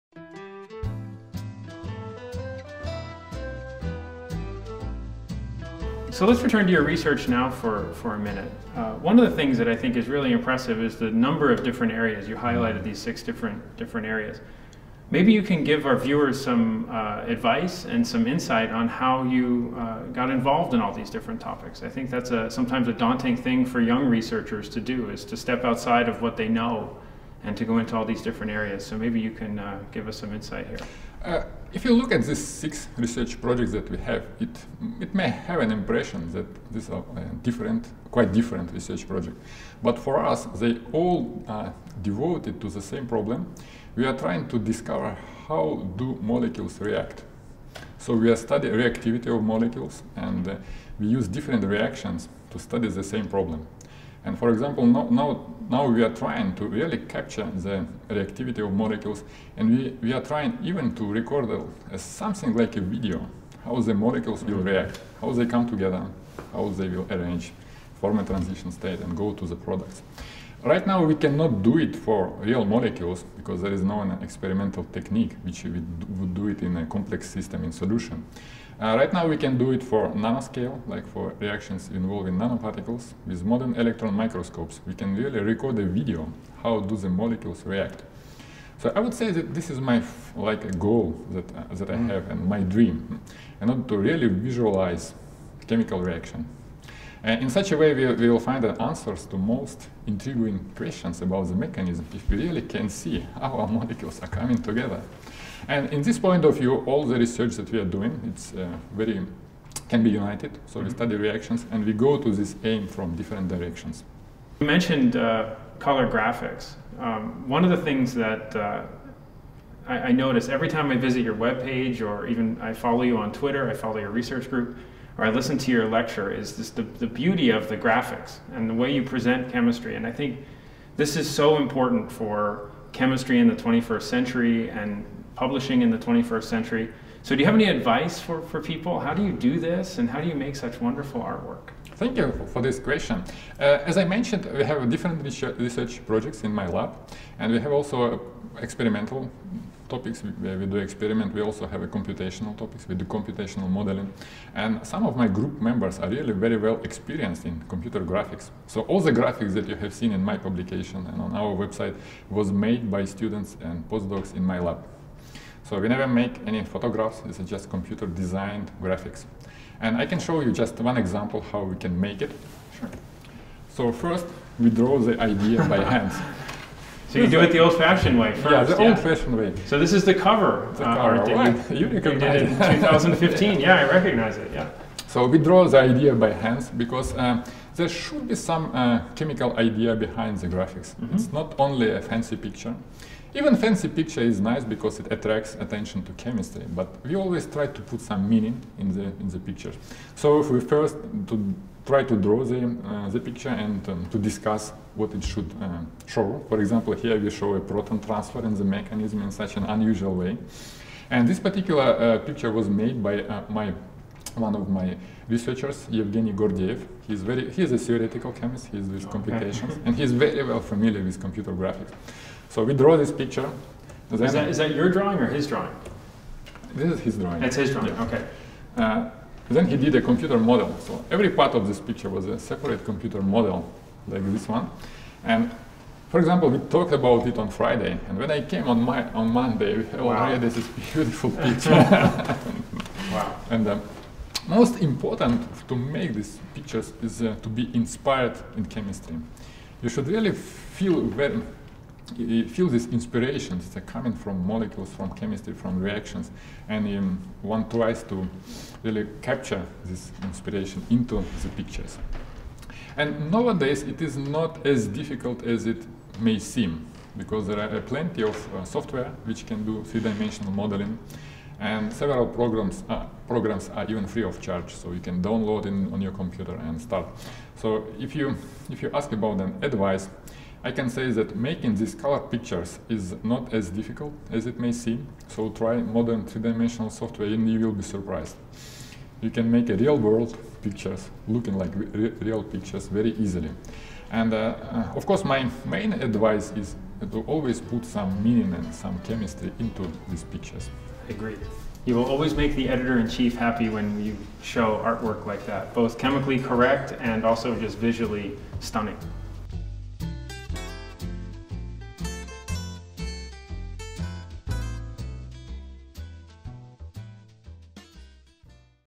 Valentine's Advice to Researchers and Authors
An interview with Valentine P. Ananikov, Ph.D., featuring Organometallics Editor-in-Chief Paul Chirik, Ph. D.